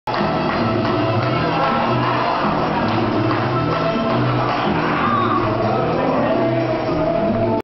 PADL BATL '07: Miki & Tim
Tim & Miki celebrate a point earned during a match.